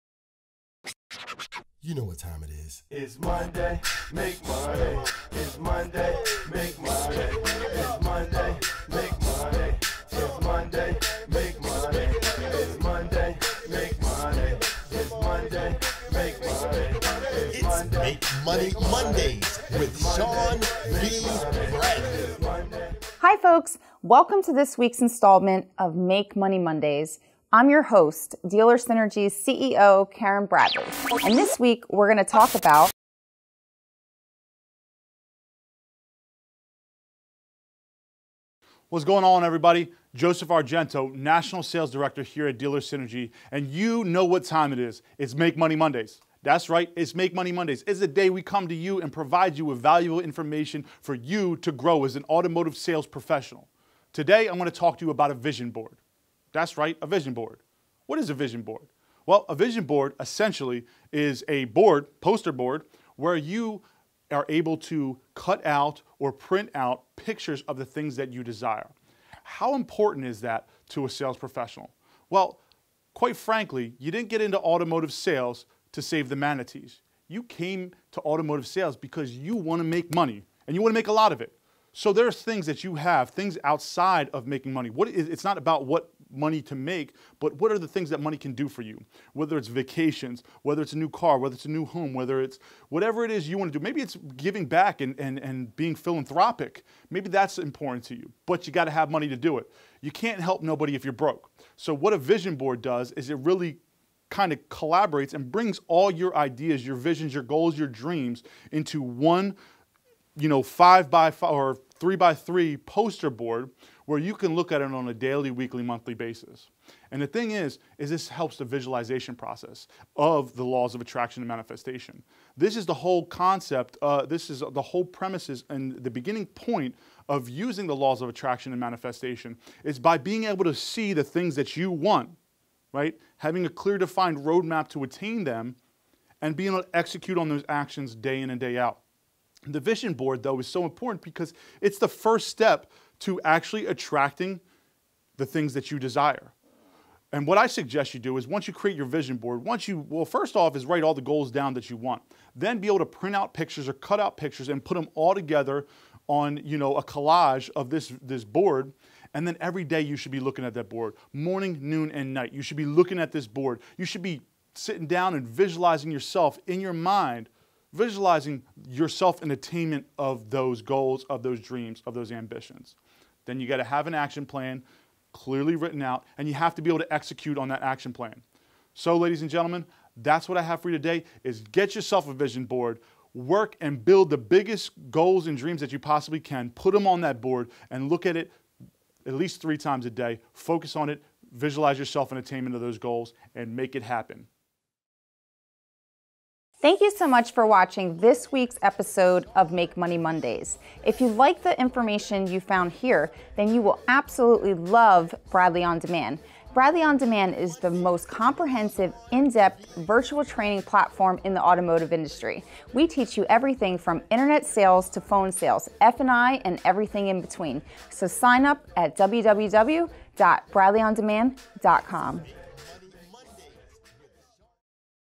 Create Your Vision Board - Make Money Mondays - Special Edition
In this week's Episode of Make Money Mondays Special Edition, Joseph Argento, National Sales Director of Dealer Synergy explains how and why you should create a vision board to help map out your success. With a vision board, you can list some of your biggest desires that will remind you what your are striving to acheive.

If you like Make Money Mondays, you will love Bradley On Demand